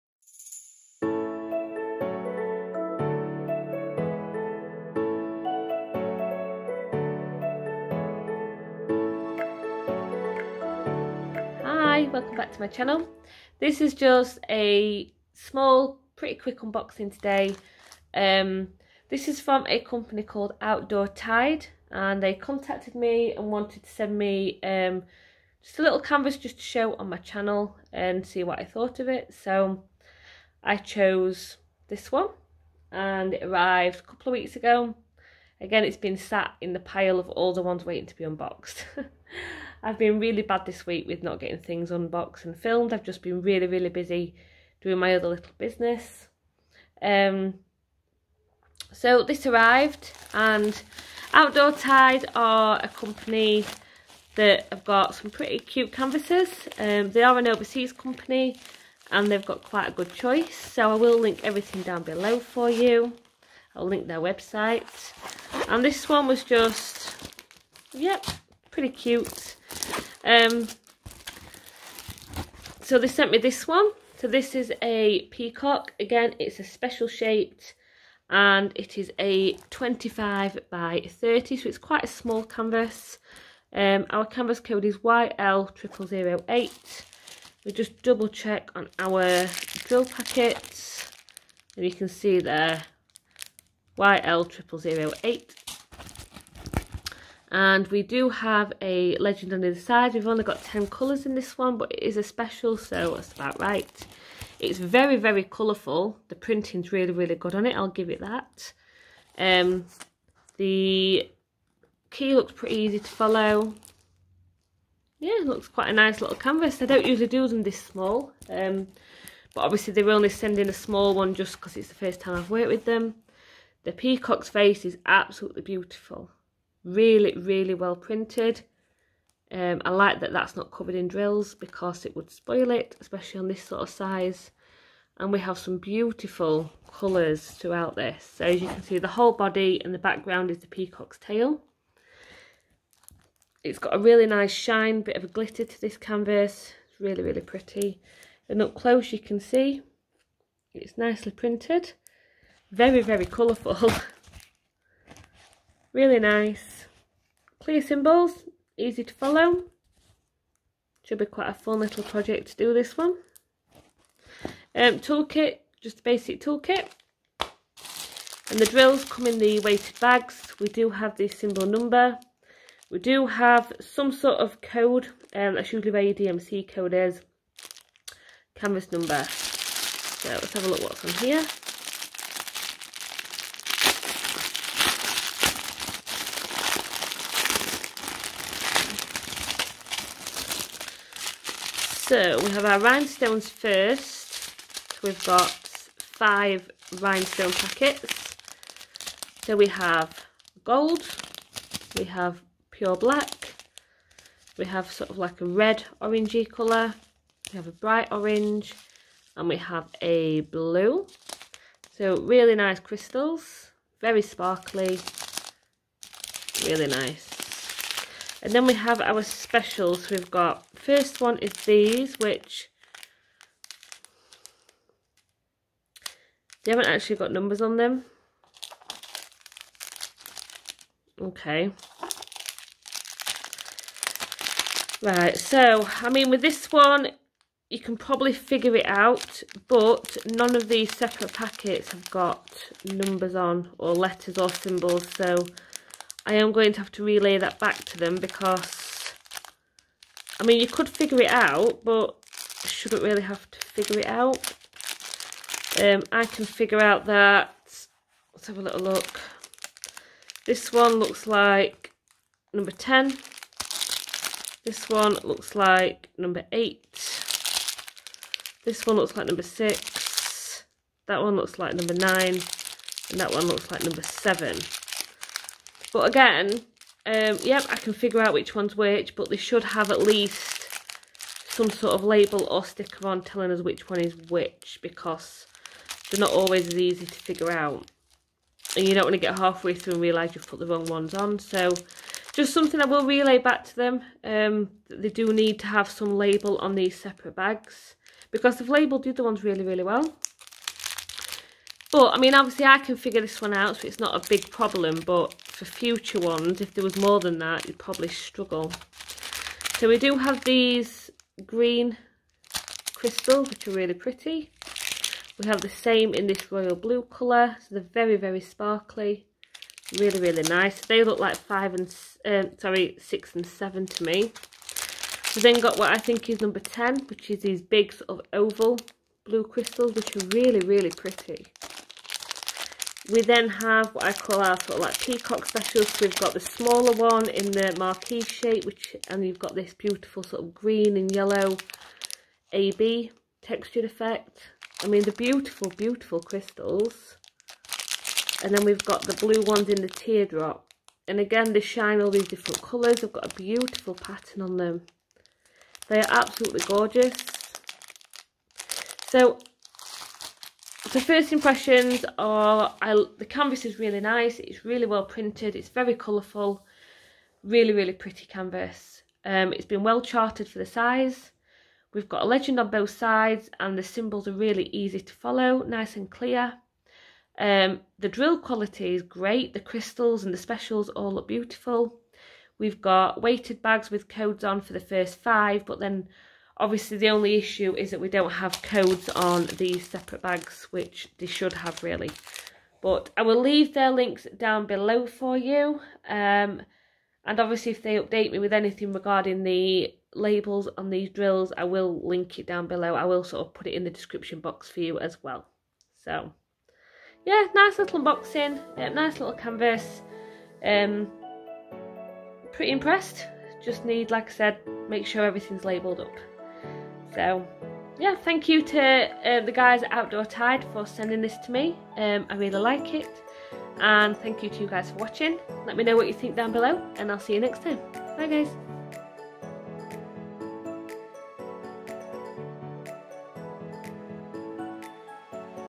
Unboxing OUTDOOR TIDE Diamond Painting
Here is my video unboxing some more beautiful diamond paintings from Outdoor Tide

🧜‍♀️ETSY SHOP (MYTHICLAY)

Link to our Facebook groups